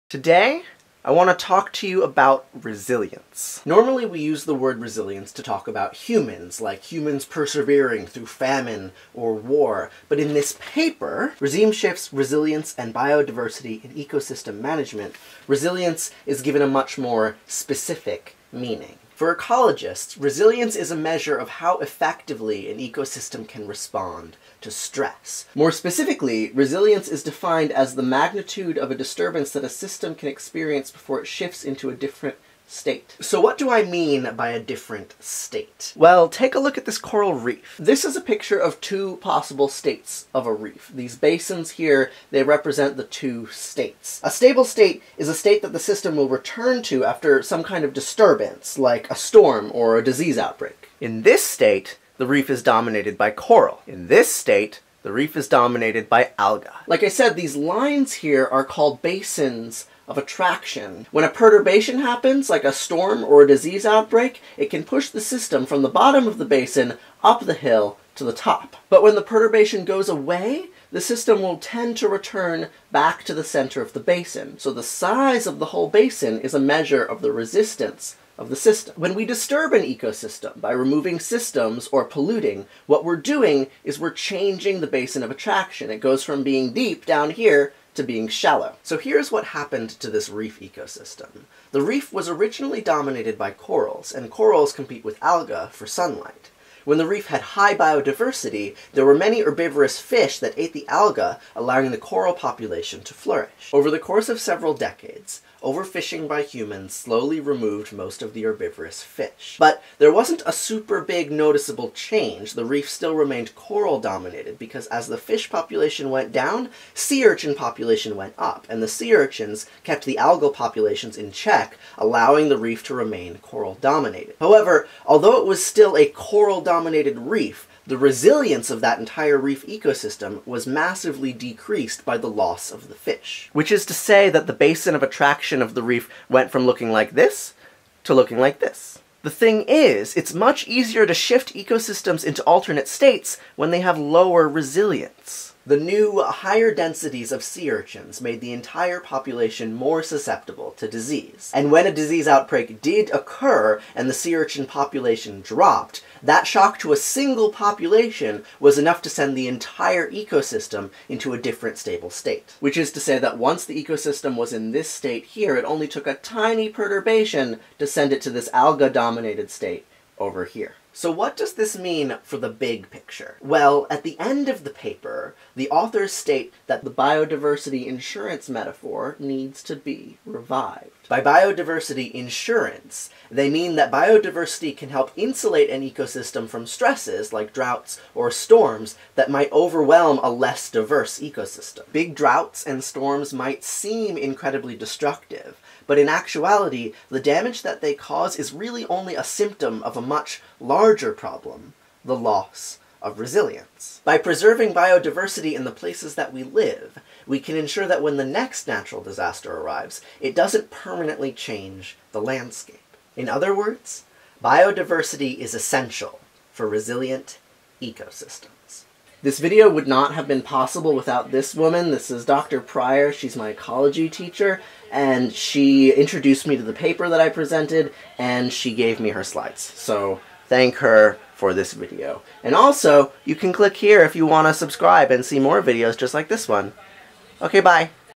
Resilience
In which Maximus describes the ecological concept of resilience as defined by the paper "Regime Shifts, Resilience, and Biodiversity in Ecosystem Management"